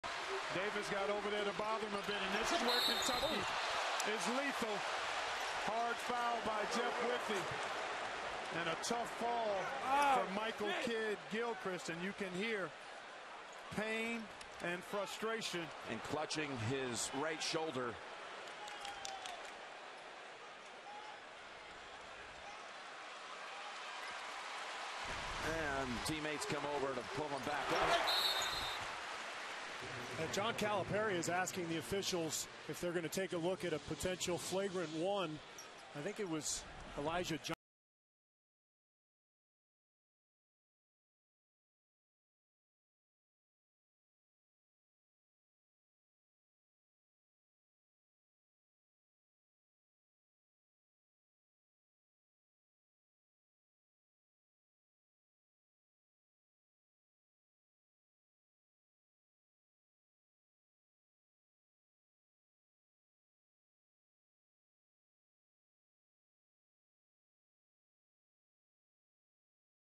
Kansas v. Kentucky: Possible Flagrant 1
A possible flagrant one during the NCAA Championship game between Kansas v. Kentucky on April 2, 2012